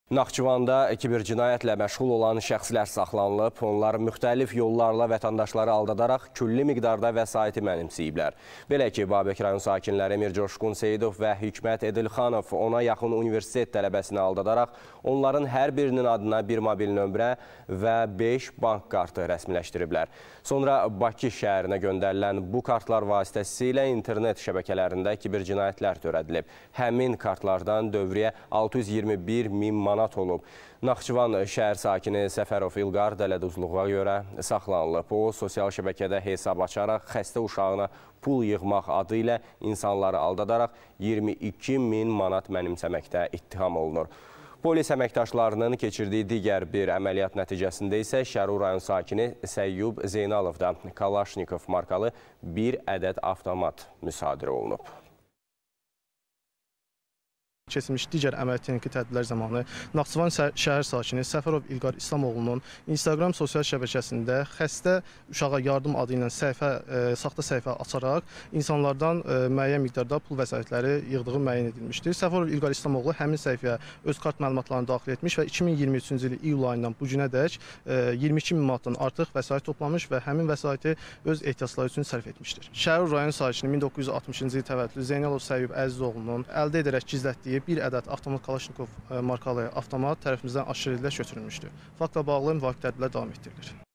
Naxçıvanda kibercinayətkarlıqla mübarizə: vətəndaşların pulunu mənimsəyən şəxslər saxlanıldı
SAXTA HESAB AÇARAQ PUL TOPLADI